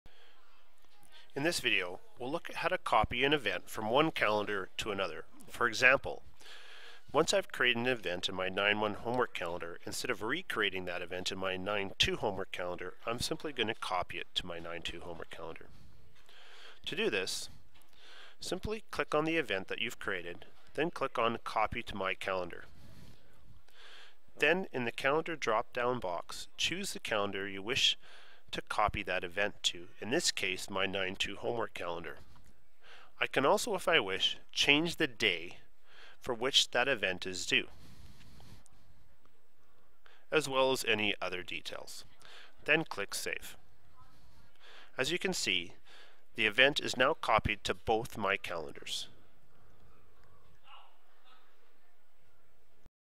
Copy calendar events
Video tutorial on how to copy a calendar event to another calendar.